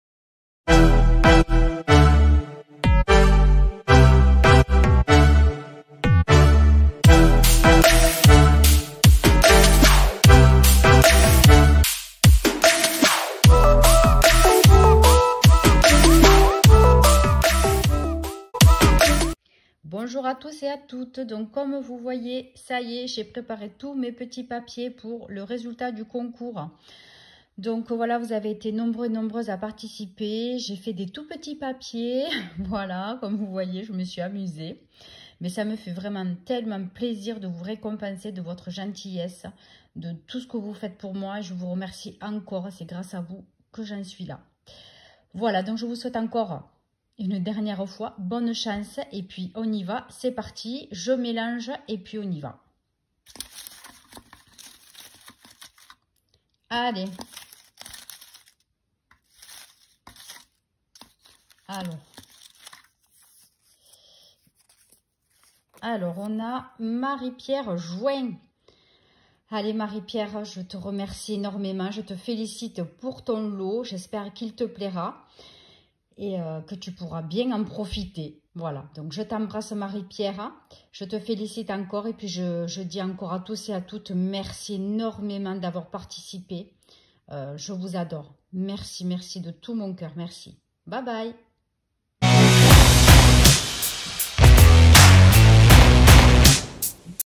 Résultat du concours des 1000 abonnés
Bonjour !! Voici le tirage au sort du ou de la gagnante du concours des 1000 abonnés !!

Merci énormément à tous les participants !
J’espère que ce lot vous plaira 😜
Pleins de bisous